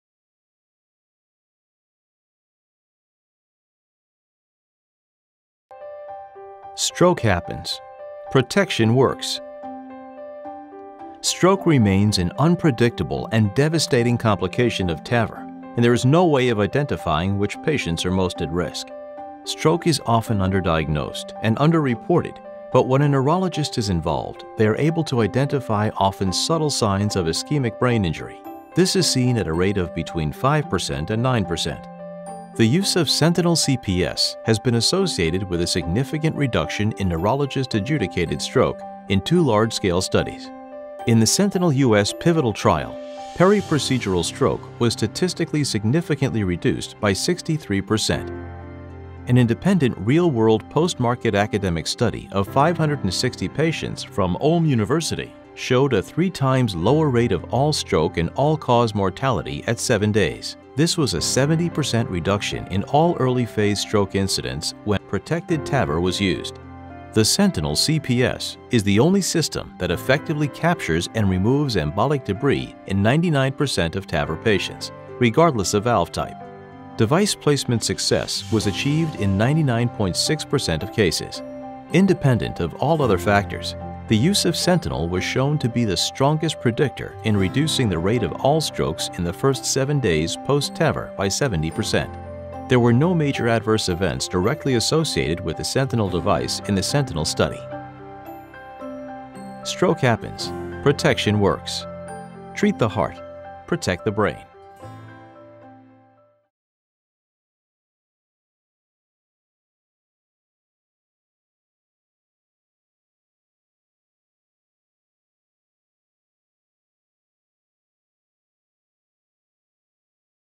Physician Whiteboard Animation: SENTINEL™ CPS Ulm Clinical Data
In an independent, real-world, post-market academic study of 560 propensity matched patients from Ulm University, SENTINEL ™ CPS reduces TAVR stroke, and stroke or mortality rates by 70% (Seeger, et al. in press JACC September 2017).